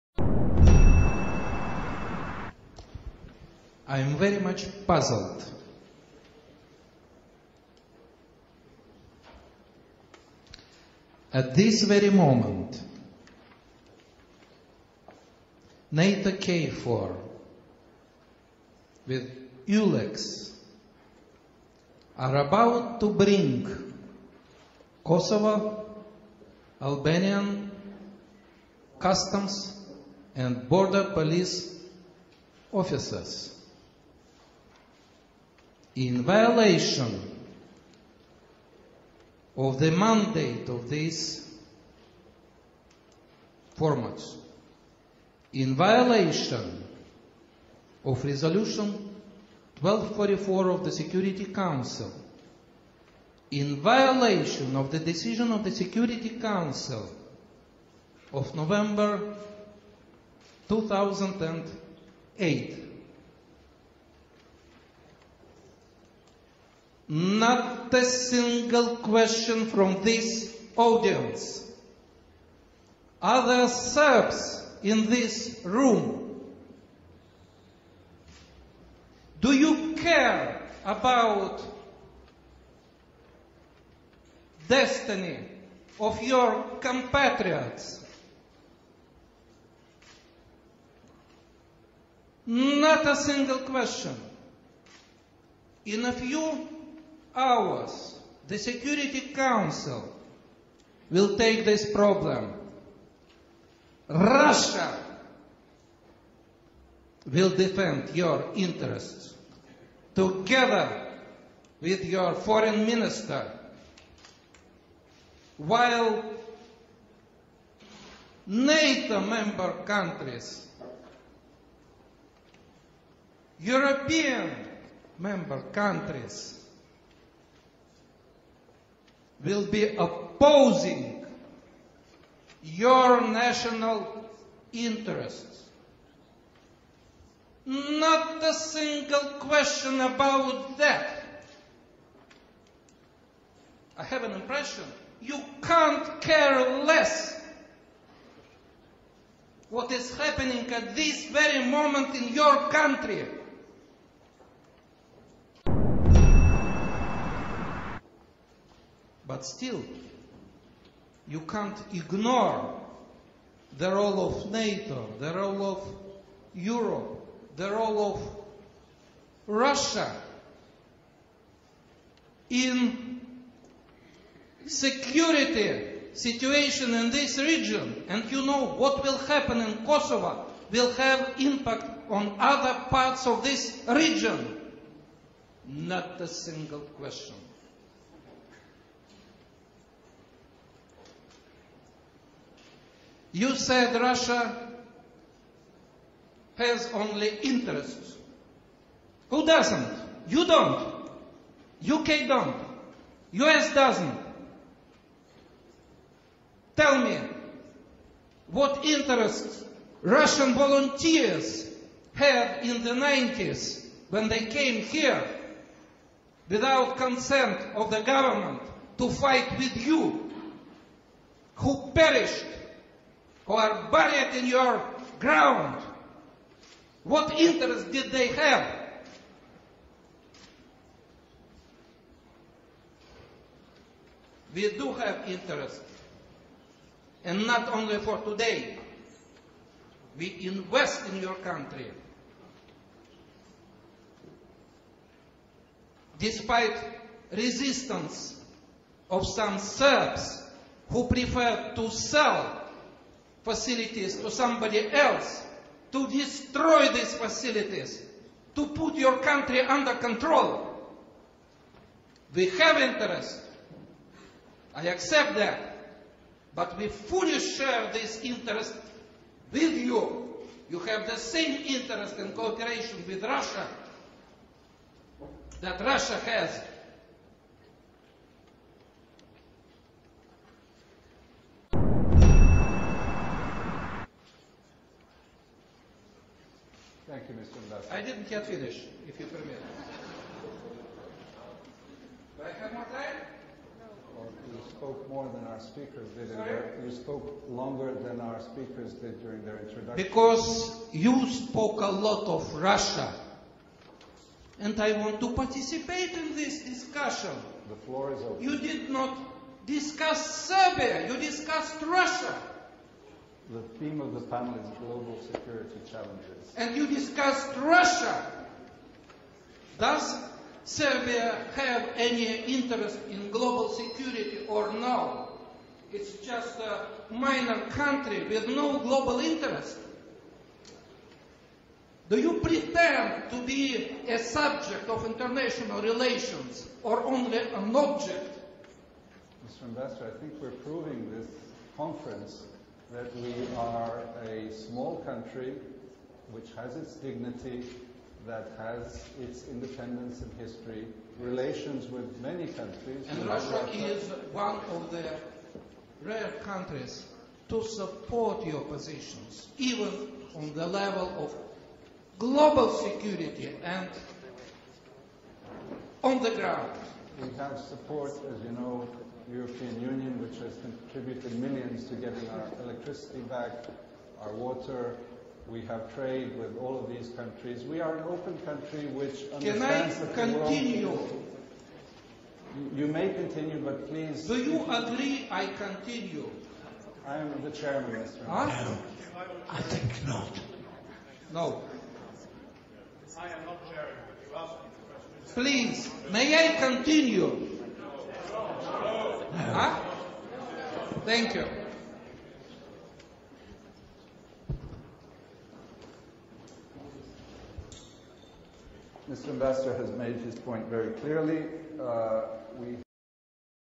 Are there Serbs in this room? (русские субтитры)
Скандальное выступление Посла России в Сербии А. Конузина на Международном форуме по безопасности в Белграде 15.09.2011. Сюжет агентства tanjug.rs

Помочь каналу рублём:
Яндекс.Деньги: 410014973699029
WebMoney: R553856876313